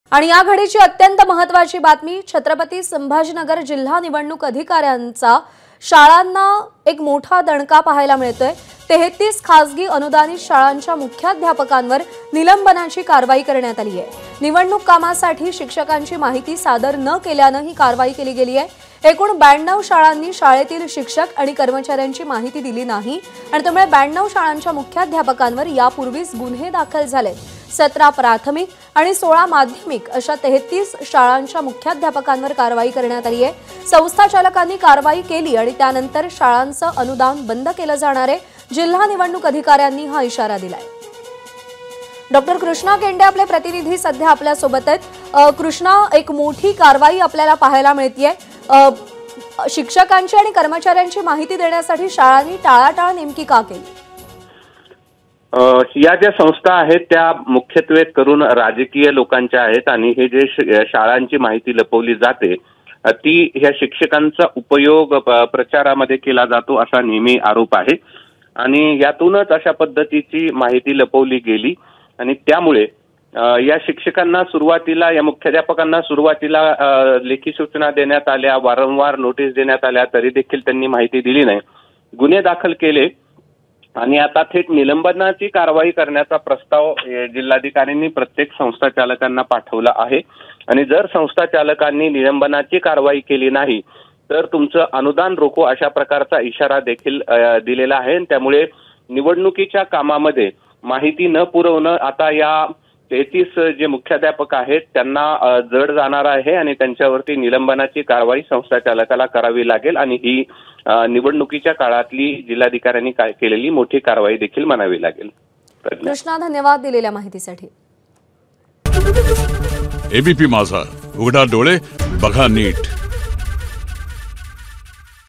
Chatrapati Sambhajinagar : ३३ खासगी अनुदानित शाळांचे मुख्याध्यापक निलंबित
छ.संभाजीनगर जिल्हा निवडणूक अधिकाऱ्यांचा शाळांना दणका, ३३ खासगी अनुदानित शाळांच्या मुख्याध्यापकांवर निलंबनाची कारवाई, निवडणूक कामासाठी शिक्षकांची माहिती सादर न केल्यानं कारवाई.

ABP Majha LIVE TV | Shinde vs Thackeray | Maharashtra Election 2024 | Mahayuti vs MVA | Marathi News